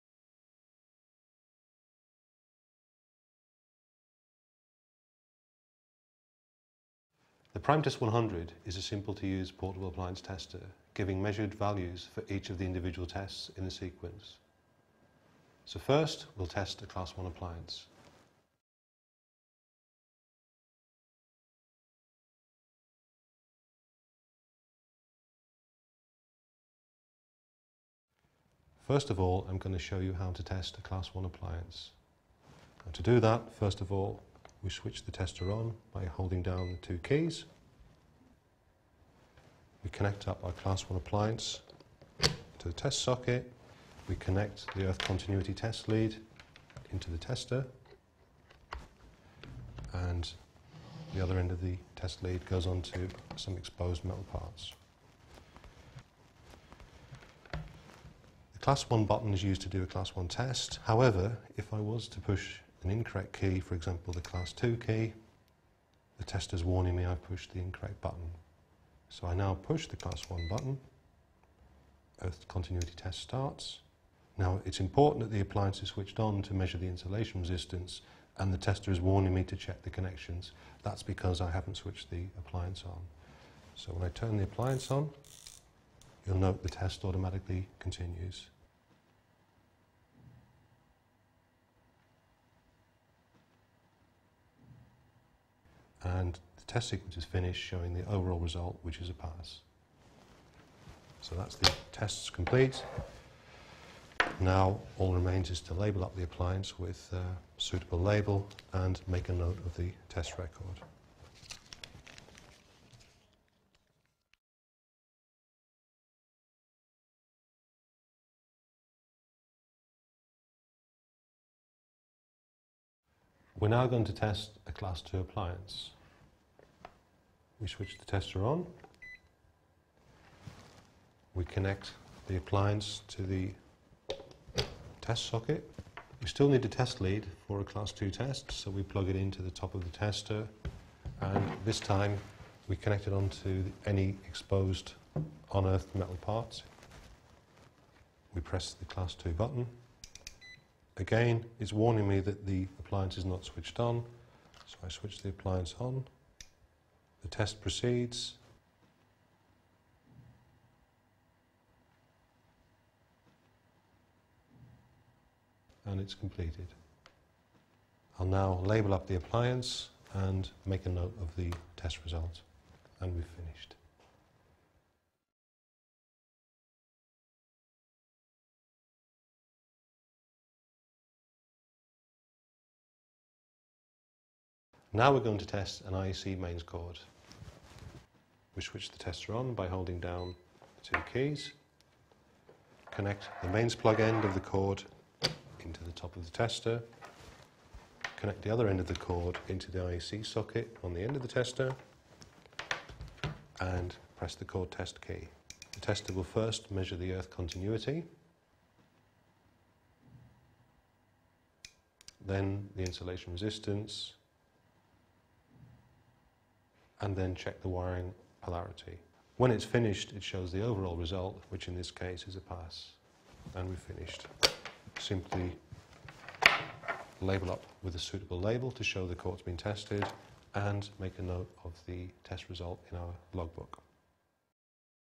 Seaward PrimeTest 100 Appliance & Socket Tester
The PrimeTest 100 is a functional and diverse manual PAT tester. Featuring three operational modes, it is an ideal choice for those looking for an easy to use, electrical safety tester